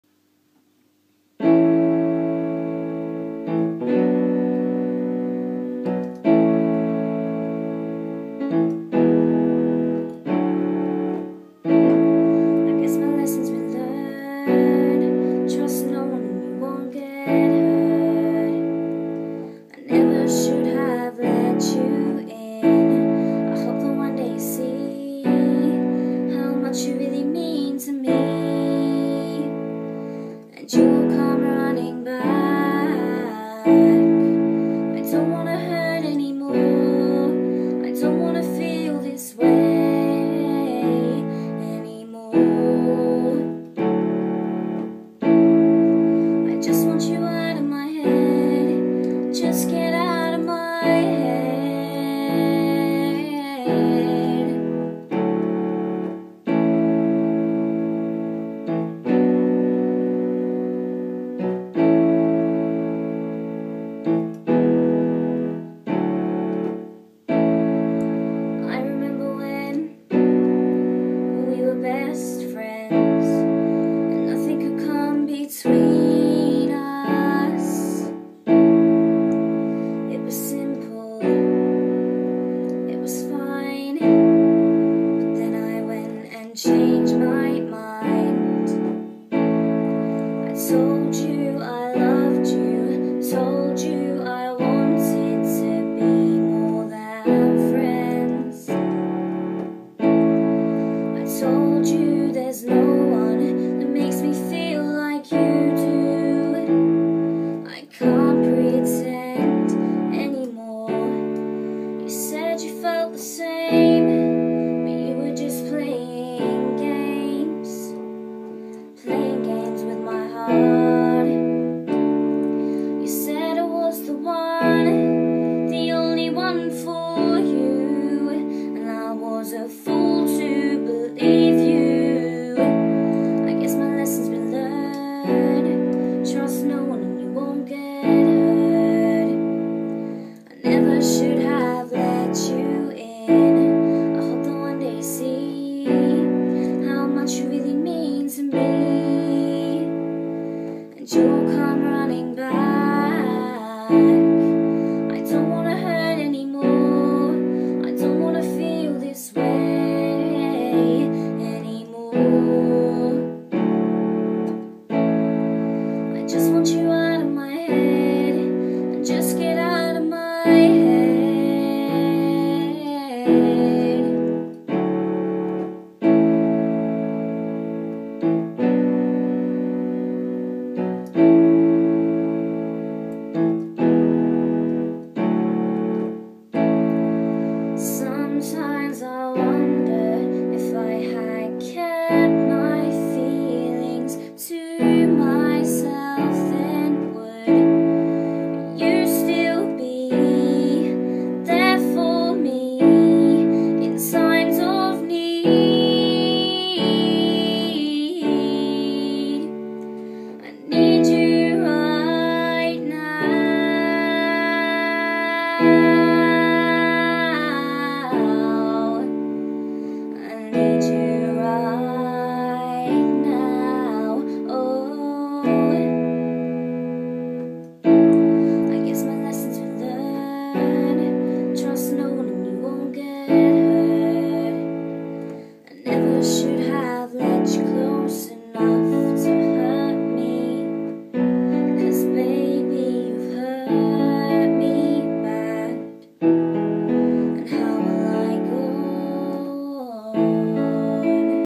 Original Song - Courtney Lentell
Well it doesnt have a name yet, and sorry for the abrupt ending but I messed it up right at the end and I couldn't be bothered to redo it again :')

Lyrics:
[chorus]
I guess my lesson's been learnt, trust no one and you won't get hurt.
I never should have let you in,
I hope that one day you see how much you really mean to me,
And you'll come running back.
I don't want to hurt anymore, I don't want to feel this way anymore,
I just want you out of my head, just get out of my head.

I remember when we were best friends,
And nothing could come between us.
It was simple,
It was fine,
But then I went and changed my mind.
I told you I loved you, I told you I wanted to be more than friends.
I told you there's no one who makes me feel like you do, I can't pretend anymore.

You said you felt the same, but you were just playing games,
Playing games with my heart.
You said I was the one, the only one for you.
And I was a fool to believe you.

Sometimes I wonder if had kept my feelings to myself then would,
You still be,
There for me,
In times of need.
I need you right now, I need you right now, oh

I never should have let you close enough to hurt me,
'Cos baby you hurt me bad,
And how will I go on?

Feedback appreciated
Just to let you know it's the first song I have ever actually completed writing, so please don't be too harsh :) xo